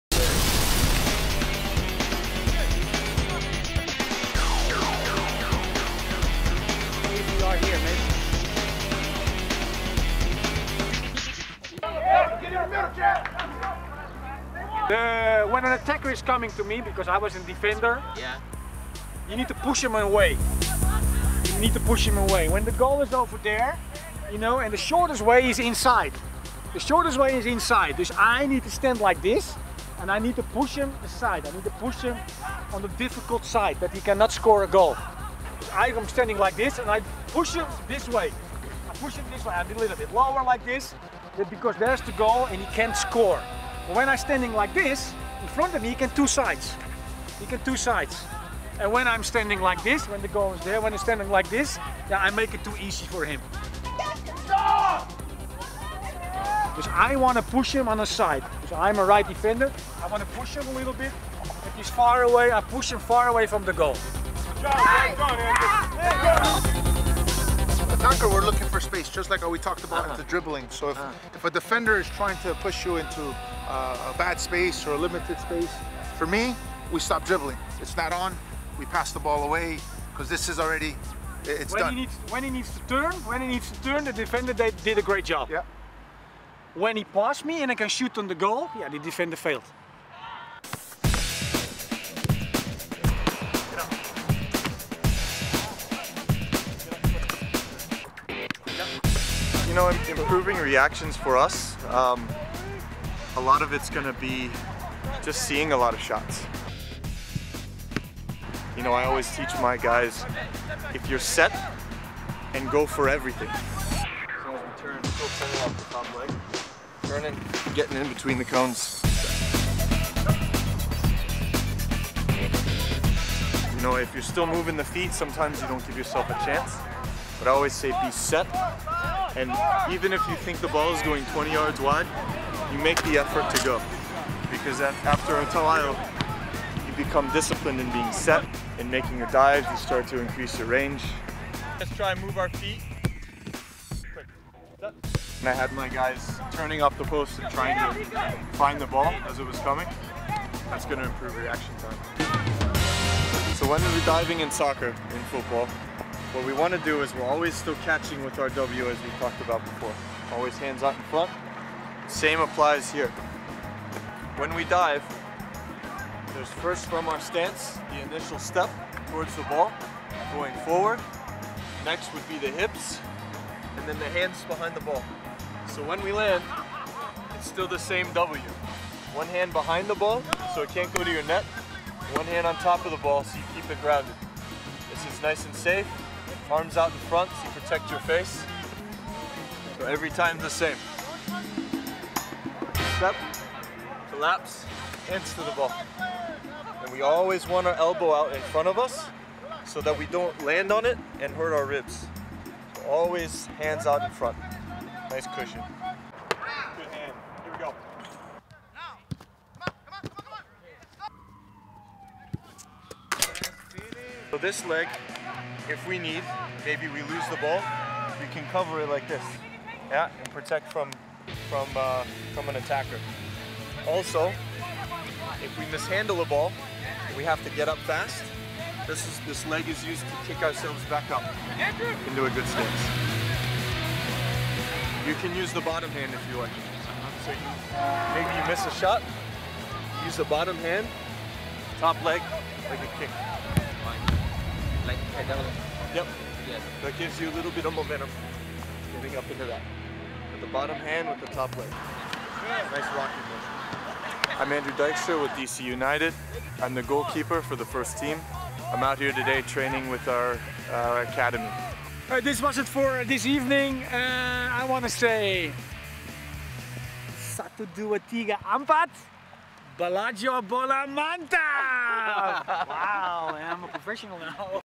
Get Into The Rhythm (Soccer Tutorial) Part 5
Let's learn how to play soccer and "Get Into The Rhythm" with Sonny Silooy, Jonathan Frias, Andrew Dykstra and DC United Academy. This part 5 would show you how to do "Defending", "Goal Keeping", and "Diving Technique".